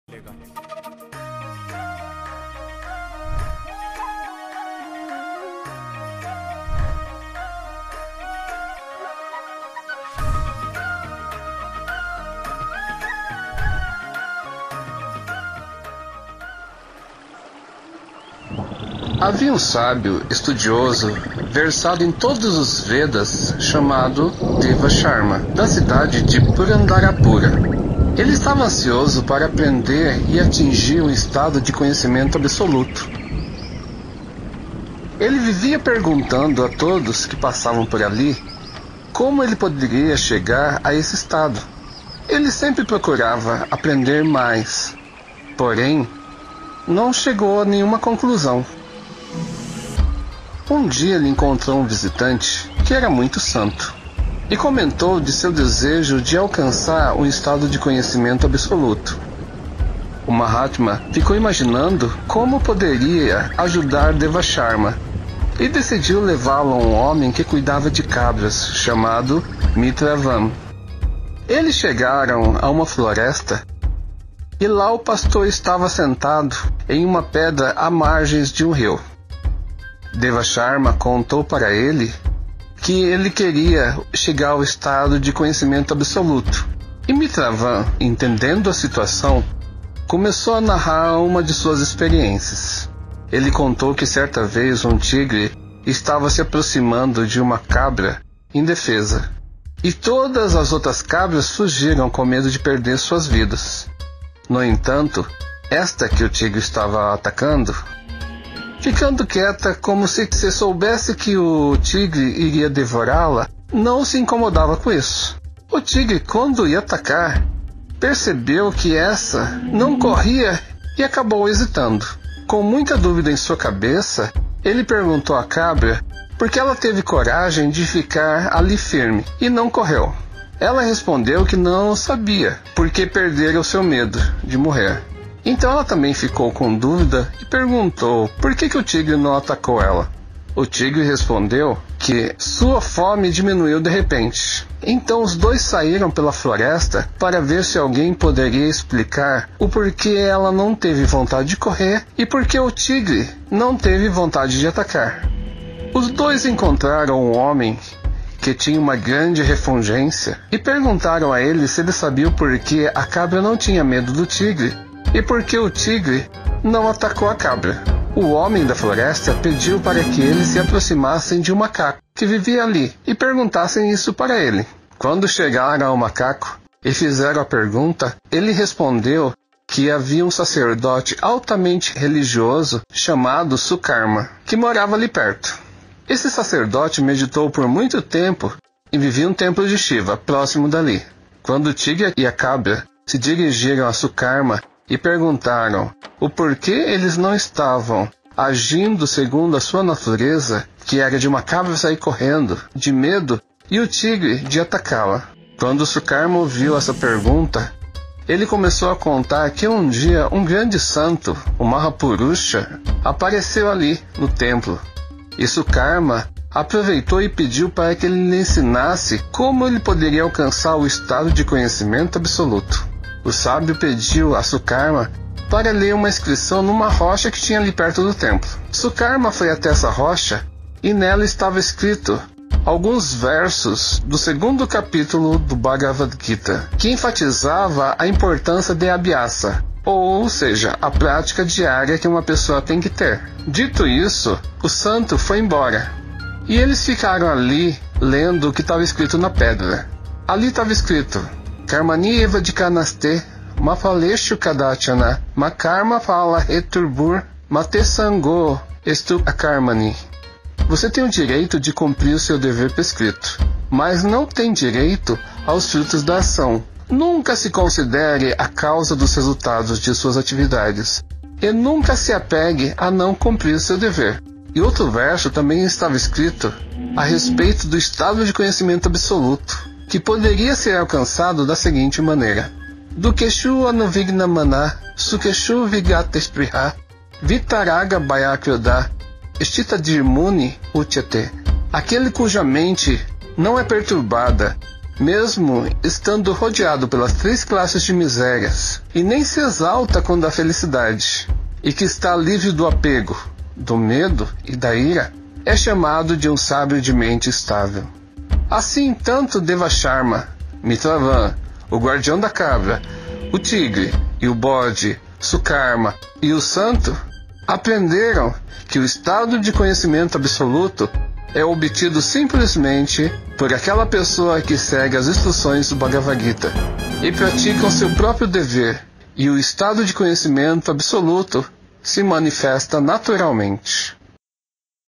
ESTADO DE CONHECIMENTO ABSOLUTO - GITA MAHATMYA 2
NESTA SEGUNDA HISTÓRIA ENCONTRAMOS UM HOMEM QUE BUSCA CONHECIMENTO, UMA CABRA E UM TIGRE QUE CHEGAM A UM ESTÁGIO QUE TRANSCENDEM A NATUREZA DO CORPO E A BUSCA DE RESPOSTAS...